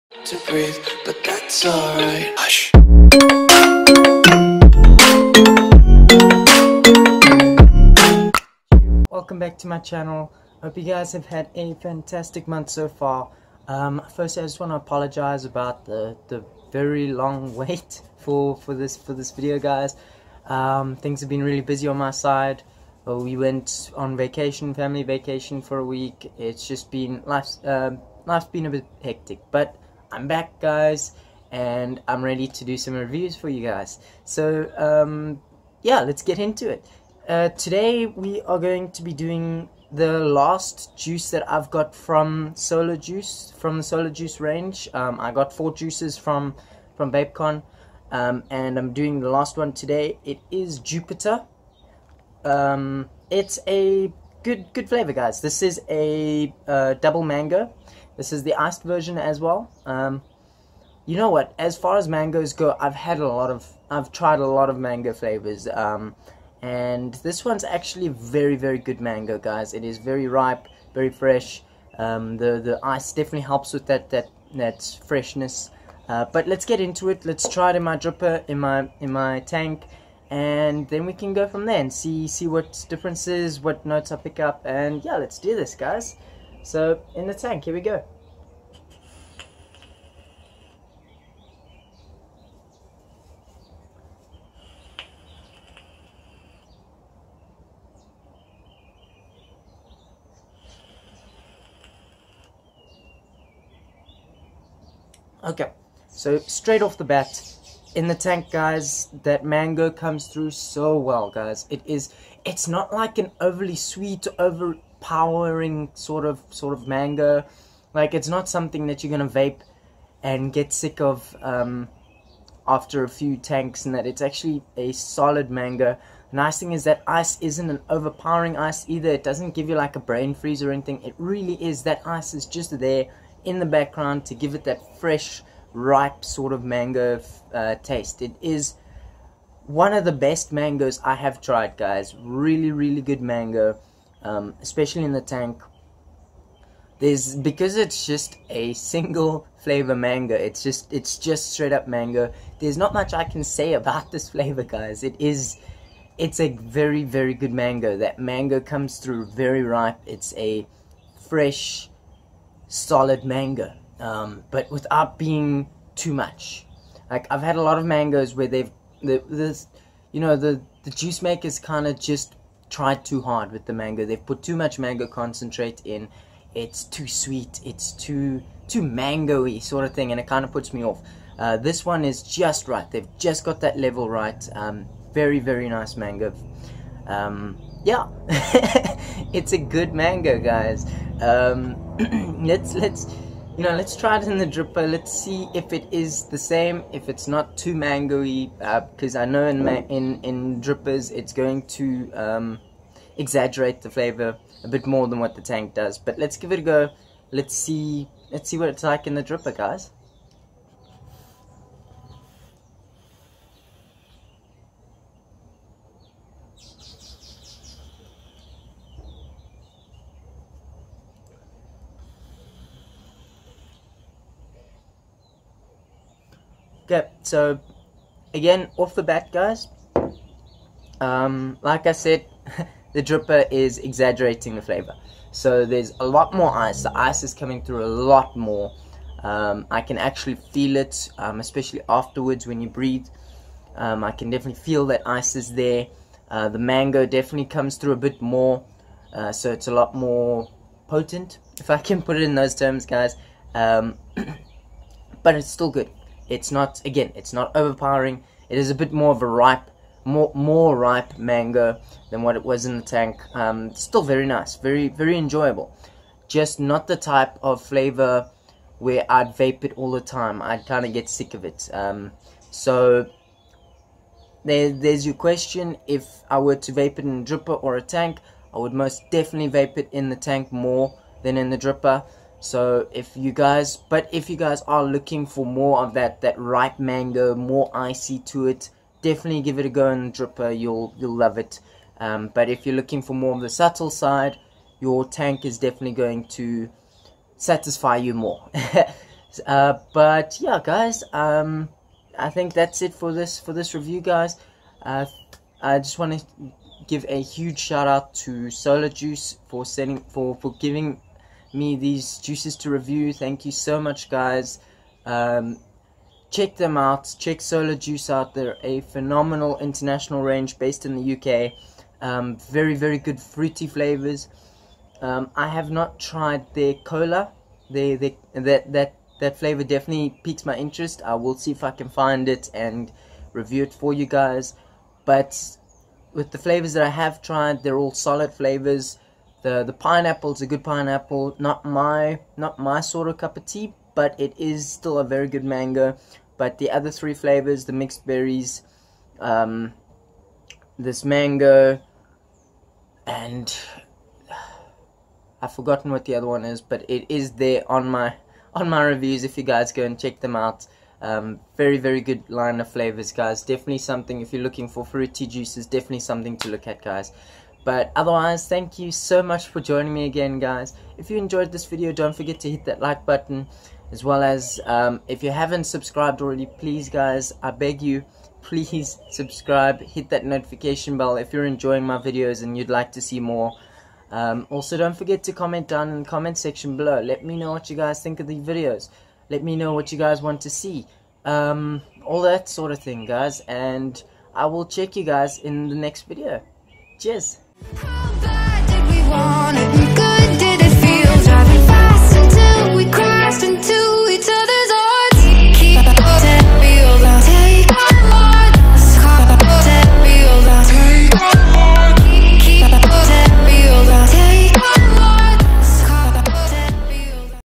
Solar Juice Jupiter Juice Review
Todays Juice Review is on Jupiter by Solar Juice.

The juice profile is a Double Mango with ice.

A huge thanks to the guys over at Solar Juice for giving me their juices to review. Be sure to check them out if yo love your fruity juices. (This is not a Paid Review)

Please note that everything said in this video is based on personal opinion and is not fact.